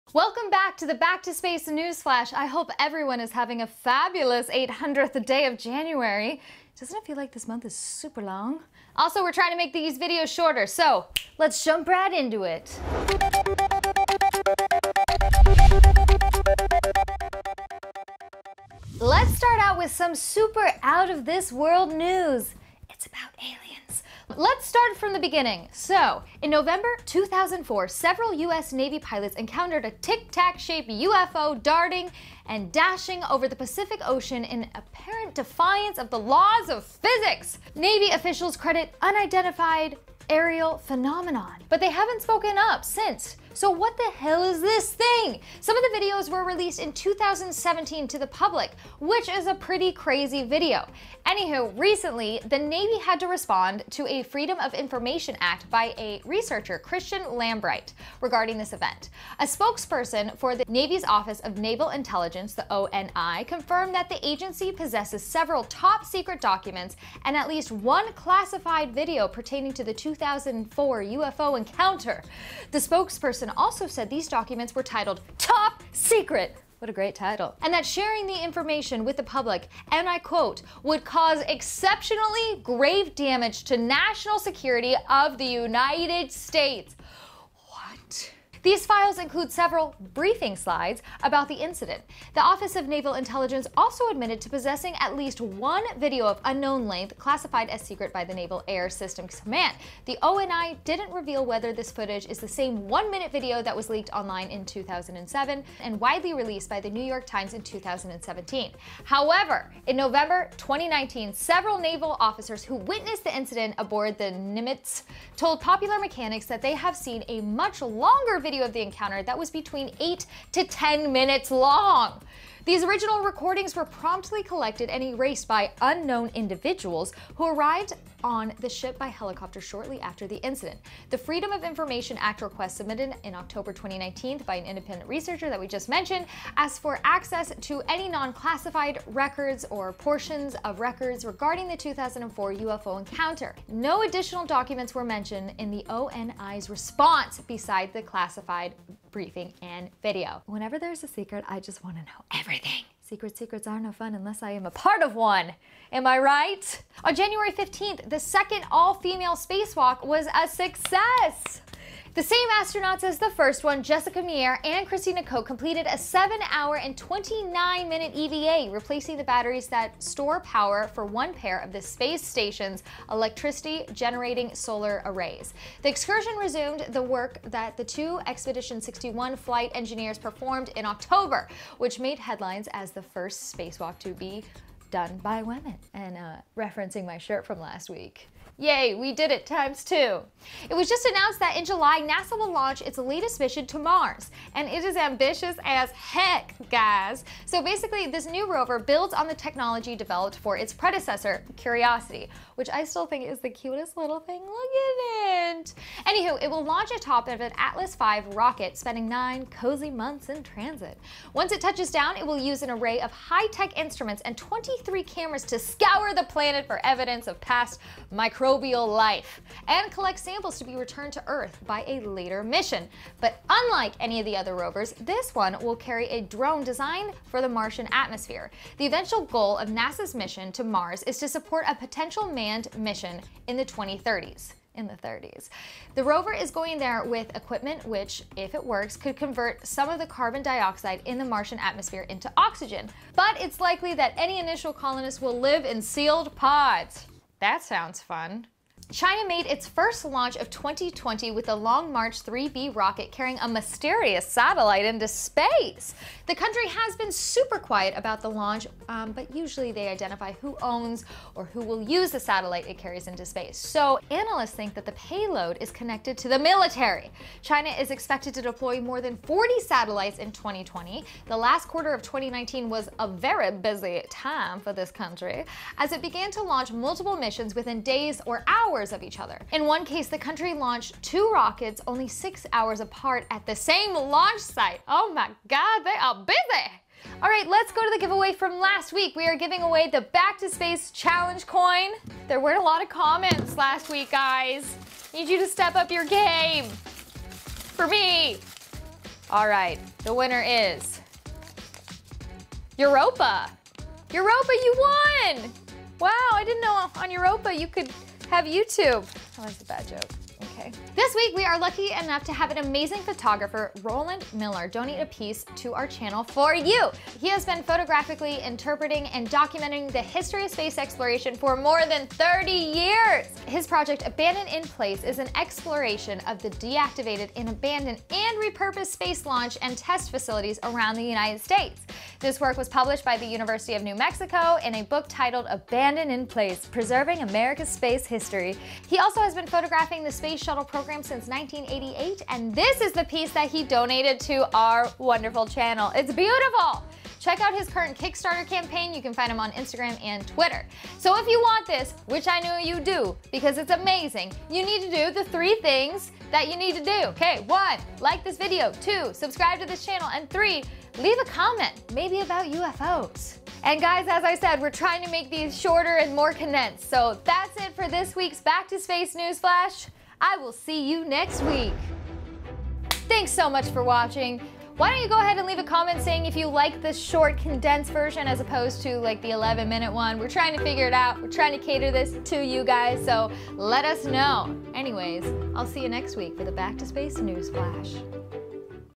Space News | UFOs, China Secrets, And Second All Female Space Walk
Contest Rules: 1.) Danielle draws winner in next video